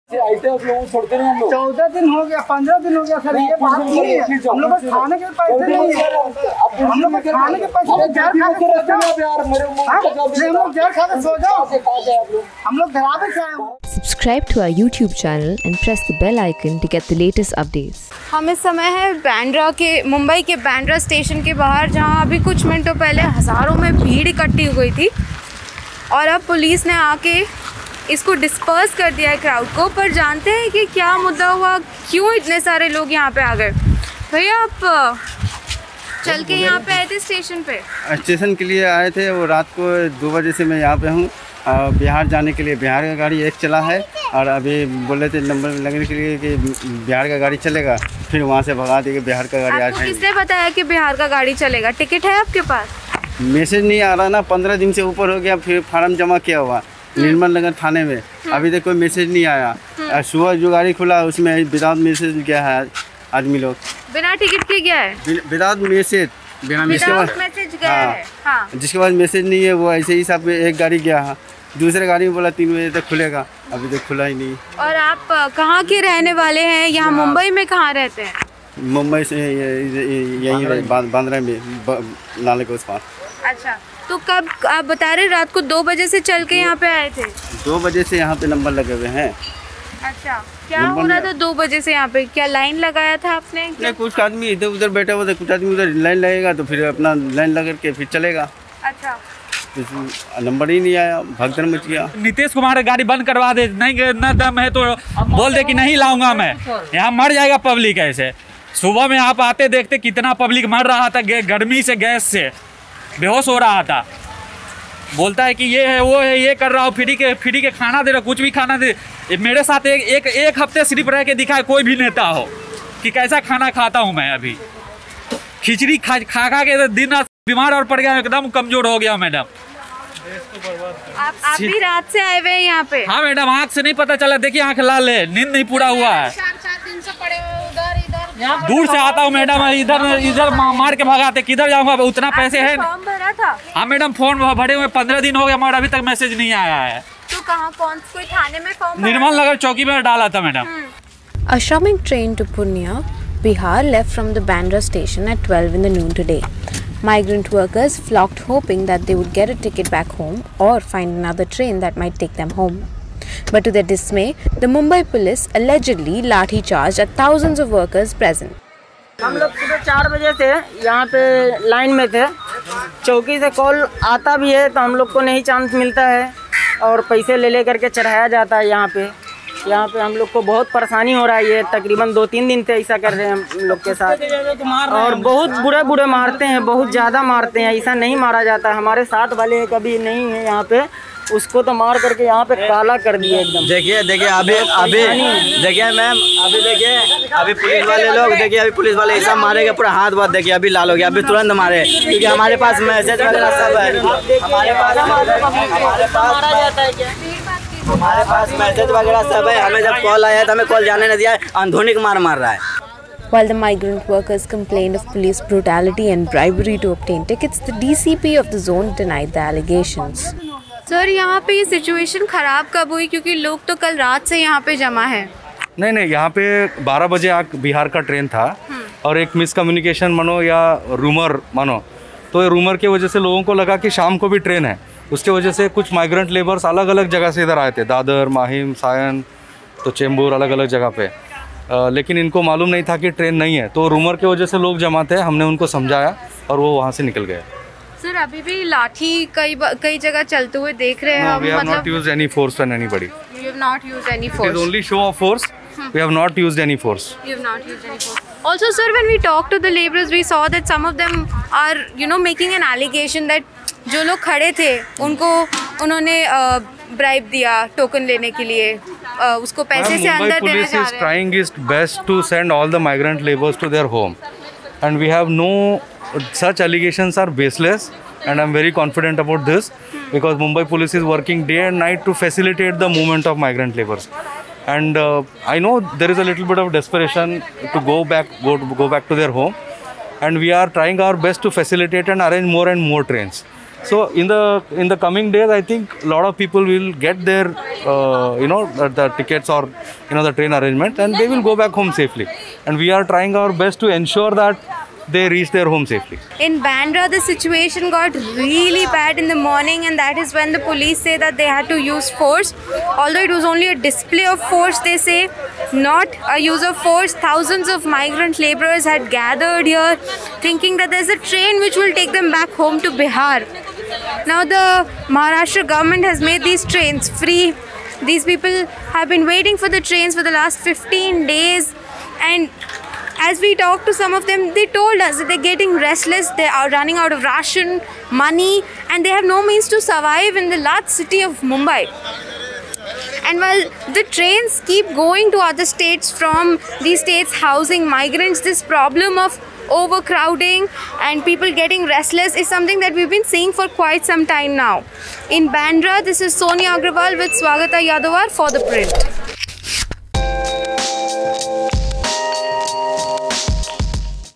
Migrant workers from Bihar overwhelm Mumbai's Bandra station in the hope of a ticket back home
As the migrant crisis in the country worsened this week, many state governments permitted special trains to take them home. News of a train from Mumbai's Bandra leaving for Purnea, Bihar Tuesday afternoon had thousands of migrant workers flocking the station and being lathi-carged by the police. ThePrint's Soniya Agrawal and Swagata Yadavar report